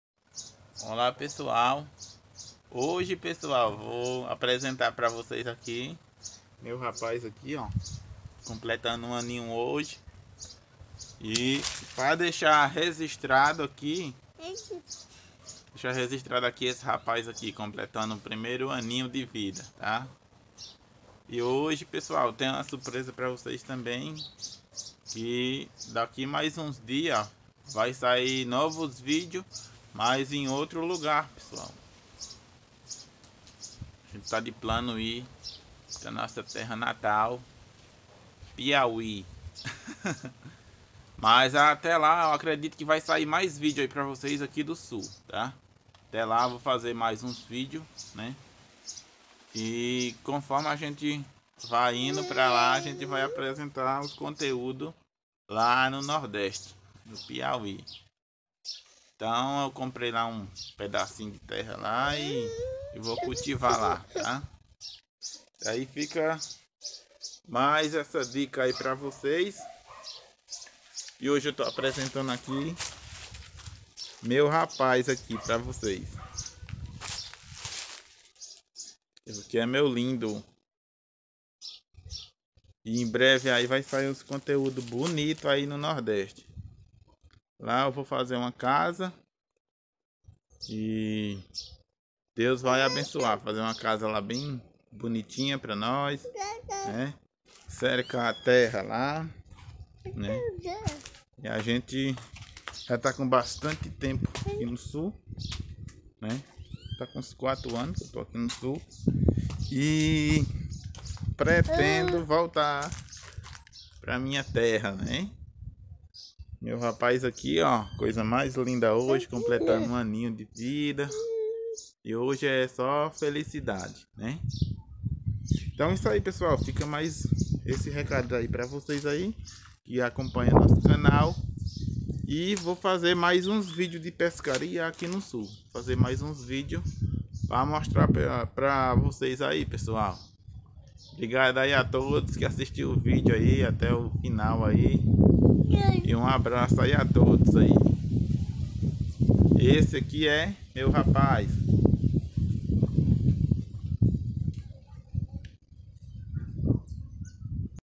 Da cidade para o Mato ( Mudanças) e 1 Ano do meu filho!
Daqui a uma semana estarei mudando do Estado do Rio Grande do Sul para o interior do Piauí

E deixar registrado o primeiro ano de vida do meu bebê